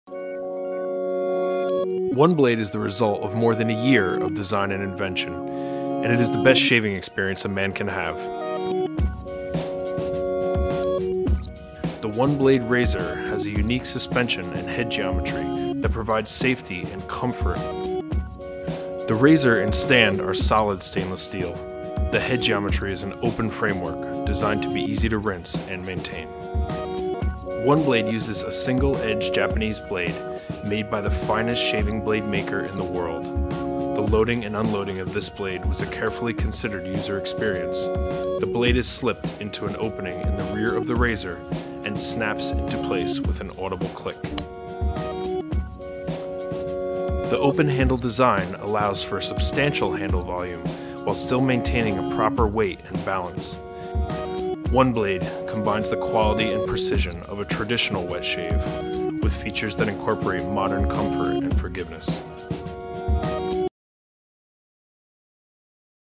OneBlade Razor: The Design Story
The story behind the innovative, boundary-breaking design of the OneBlade® Razor – traditional shaving at its best, fused with 21st century cutting-edge technology.